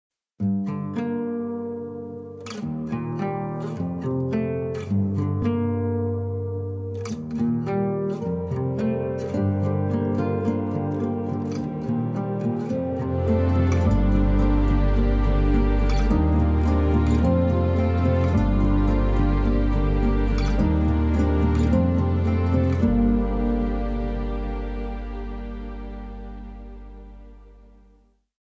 Bat Wing Spur Straps
Hand made bat wing style spur straps.
Made by my brother Keith. Great quality leather work.

Promotional video shot by us.